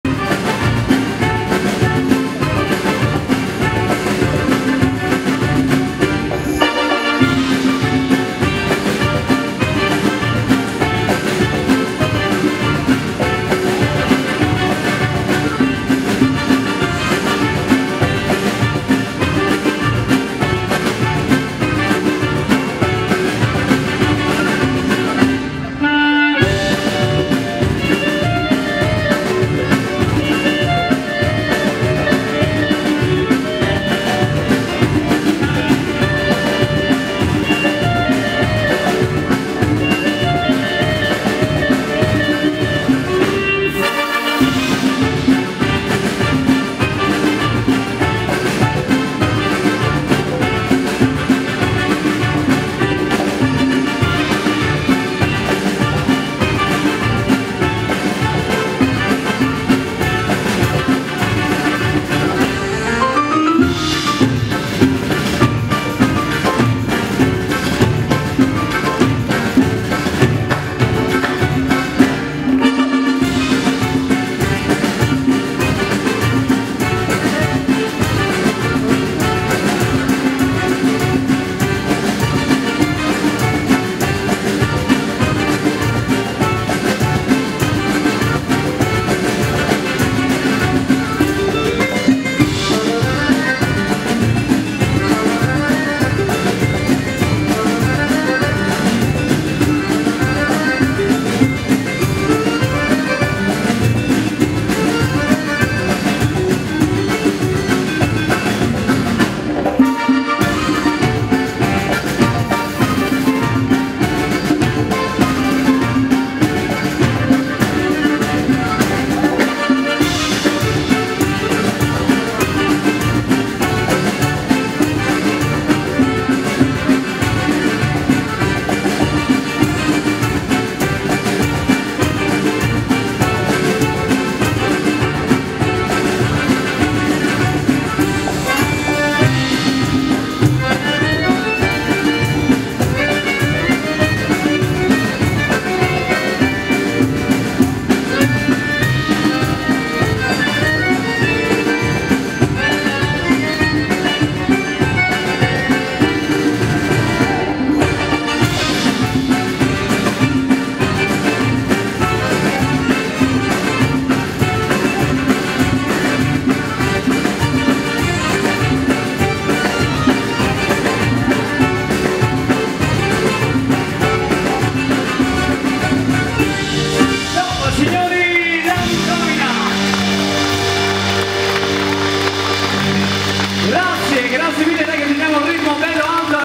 Gianmarco Bagutti e Federica Cocco al ballo
Gianmarco Bagutti e Federica Cocco che ballano Taverna Verde Forliì 20 ottobre 2013